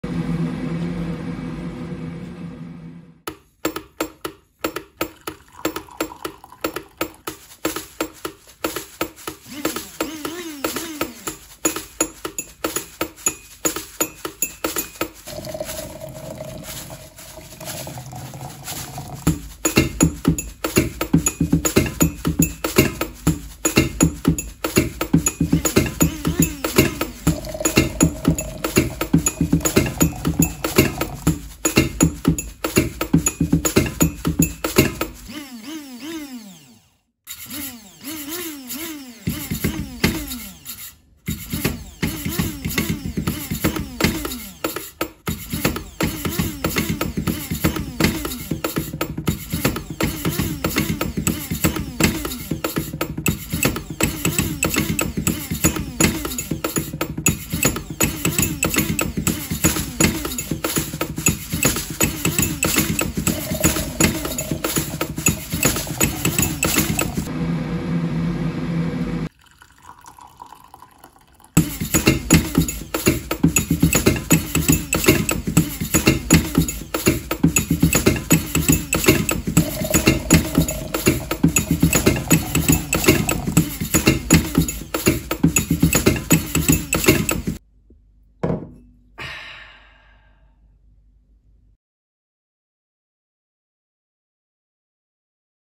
Coffee Percussion - Song made whilst making myself a coffee!
I'm a drummer that likes to make beats whilst doing every day activities. I drum on random objects and things I found around the house. I love to see what sounds and beats I can get out of everyday household items.

Rhythm is Everywhere